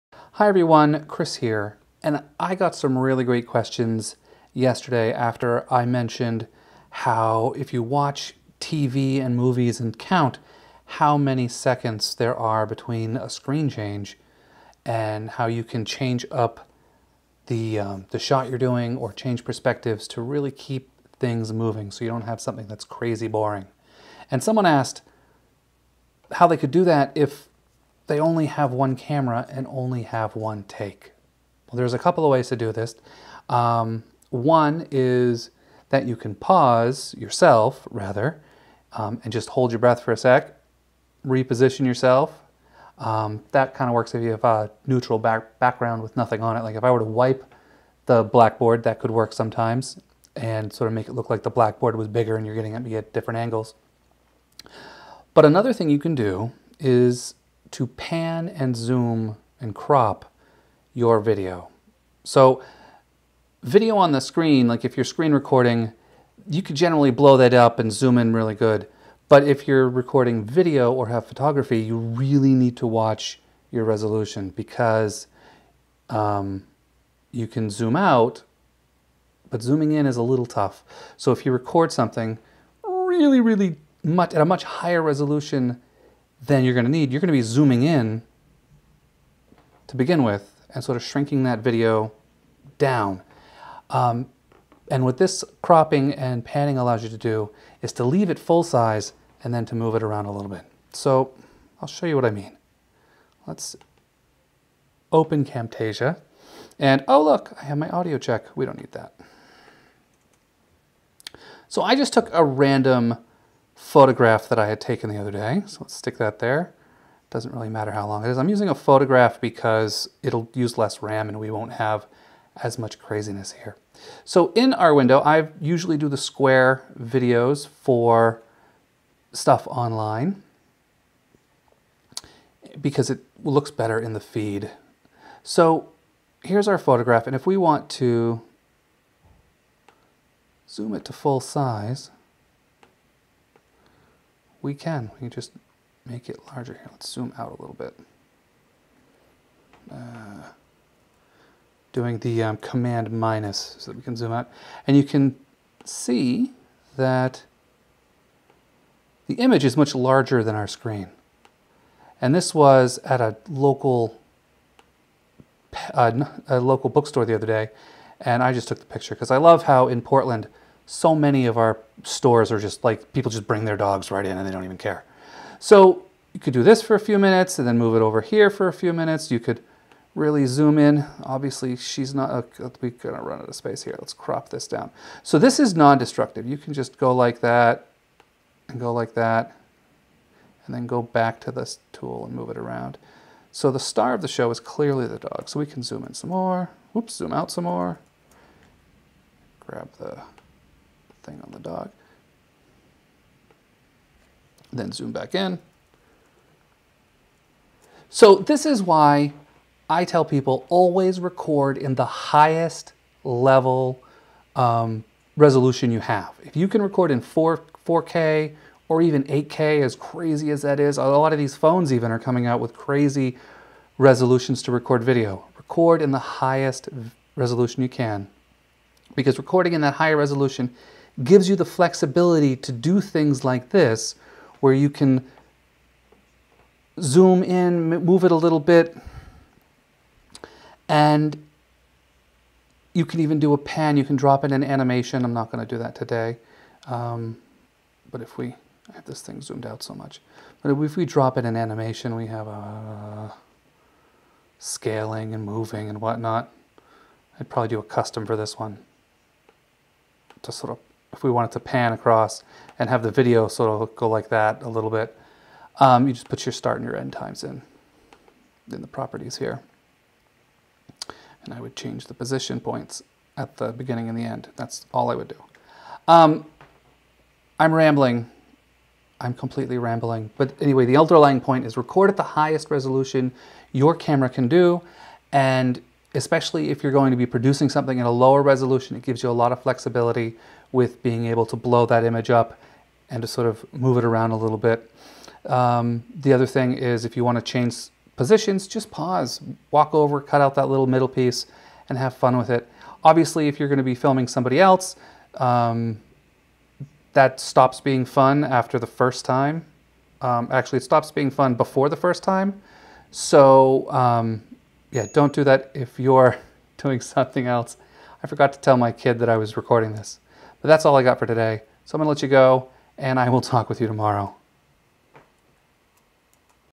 Why you should record at the highest resolution
When making instructional video there is the risk of just having a talking head, and yes I see the irony there, but there can be some ways to take the long video and make it a bit more dynamic and those options come from multiple cameras and mixing up some of the shots.  But what can you do if you only have one camera and maybe only have time for a single take?  My quick and dirty trick is to record at a much higher resolution than you are going to need and a bit wider than you would like.  You can "zoom" in using your video editing software in post-production.  The key though is you should only "zoom-in" to the actual resolution of the video so if you have recorded at twice the resolution of your final project then you can zoom in 2x but you don't want to do more than that our your video will look pixelated and blurry.  And you don't want to overdo the effect or it will stop working and will be a distraction.  Other visual elements can also help break up the video and keep your learners' eyes moving so they don't fall asleep.  Even something as simple as tossing in a lower third can help keep the eyes from drifting closed.